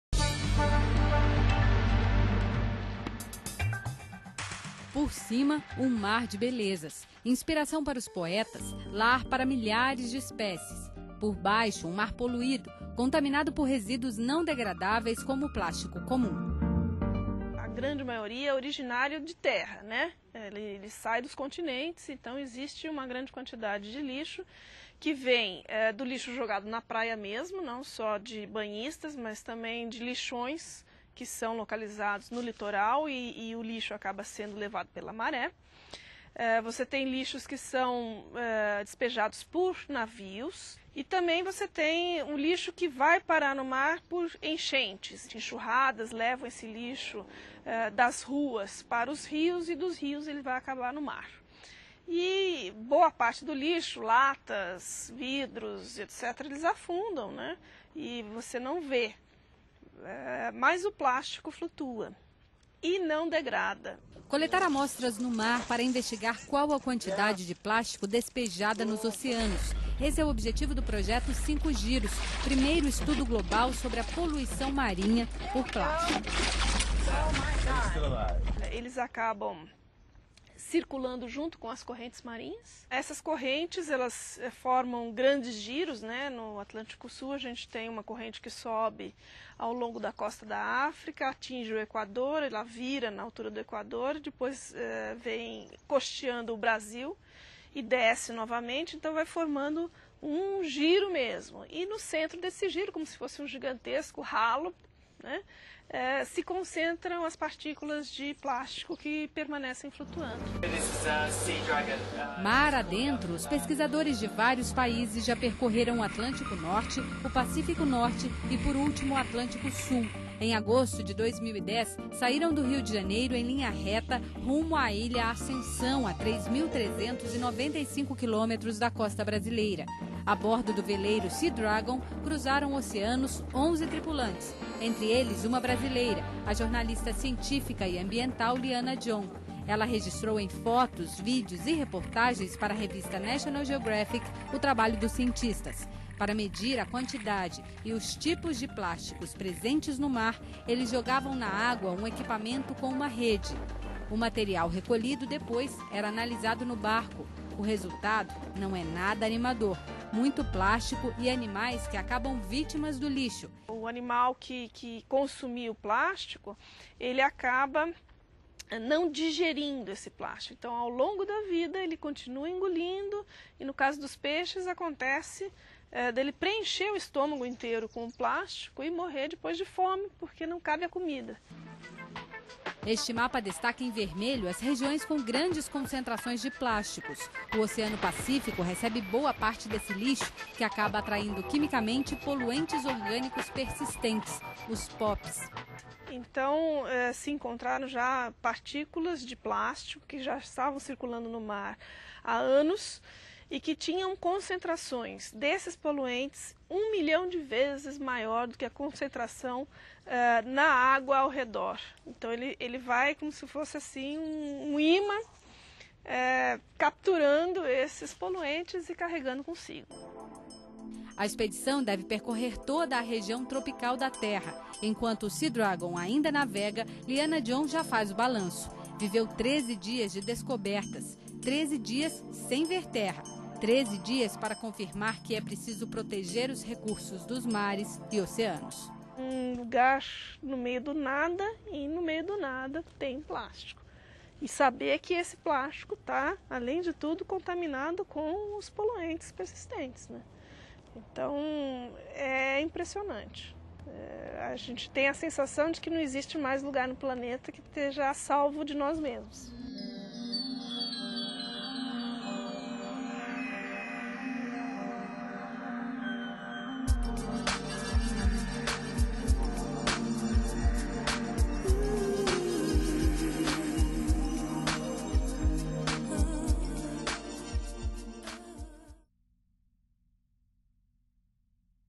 Jornalista brasileira acompanha expedição científica que faz o primeiro estudo global sobre
A jornalista ambiental Liana John percorreu mais de 3 mil quilômetros no Oceano Atlântico durante duas semanas a bordo do veleiro Sea Dragon. Ela acompanhou um grupo de cientistas do projeto 5 Giros em expedição no Atlântico Sul .Eles recolhem amostras de